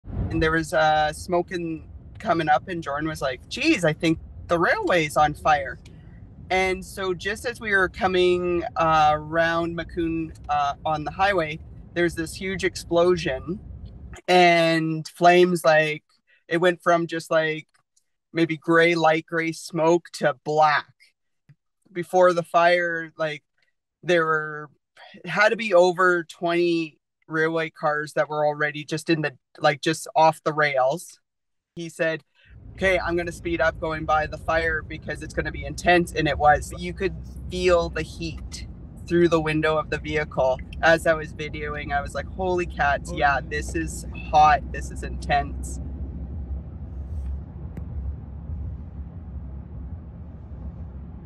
Train derailment in southeastern Saskatchewan leads to massive fire
Federal investigators are headed to southern Saskatchewan, where a train derailed just outside the village of Macoun.